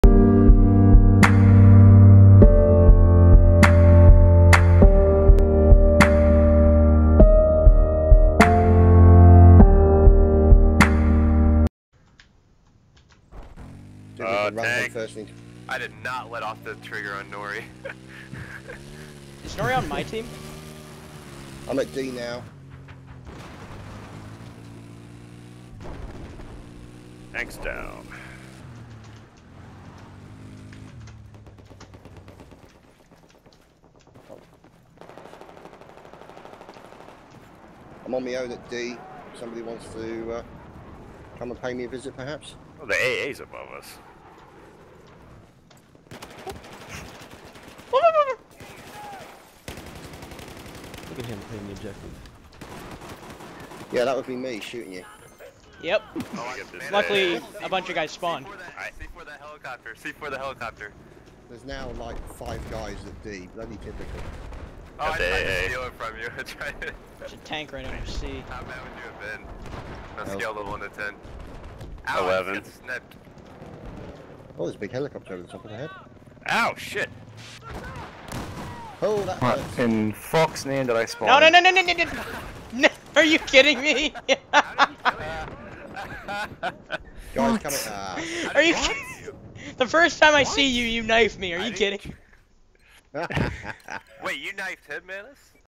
am i a pubstar now?
first spawn of the game after waiting to join my friends mantis701 is one of them...